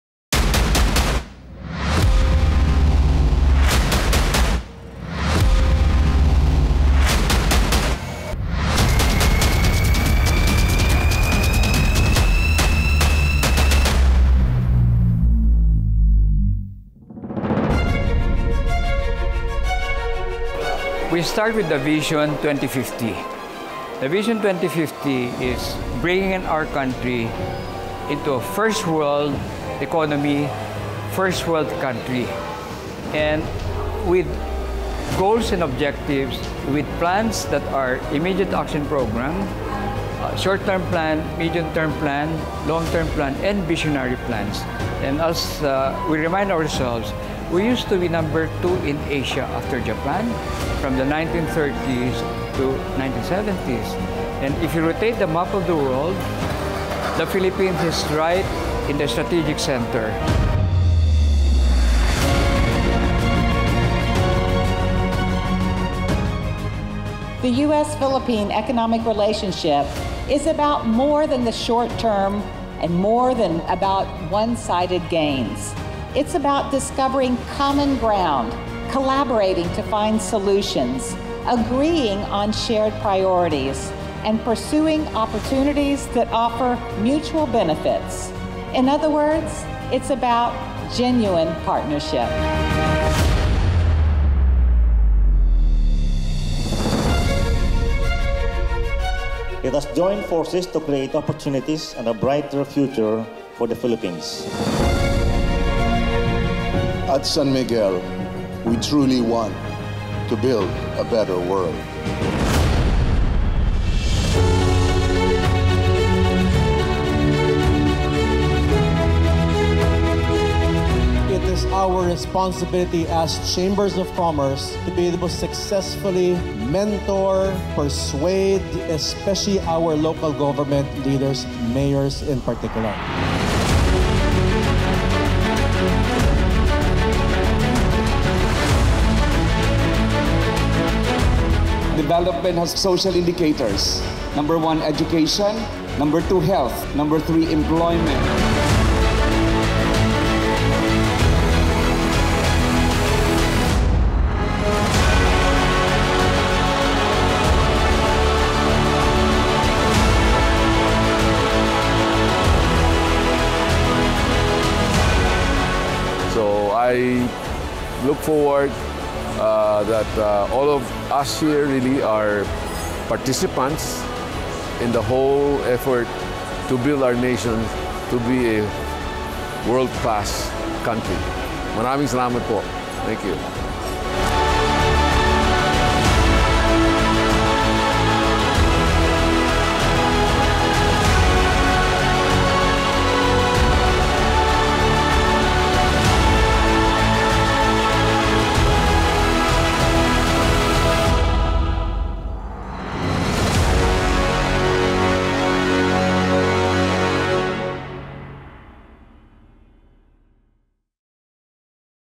49th Philippine Business Conference & Expo I Same Day Edit: Day 2
The 49th Philippine Business Conference & Expo Organizing Committee,  chaired by Architect Felino Palafox, Jr., extends its gratitude and appreciation to all the delegates, speakers, moderators, exhibitors, sponsors, and media partners for their generous support and active participation. THANK YOU!

We are pleased to share with you the video and photo highlights of the recently concluded two-day conference. Enjoy watching!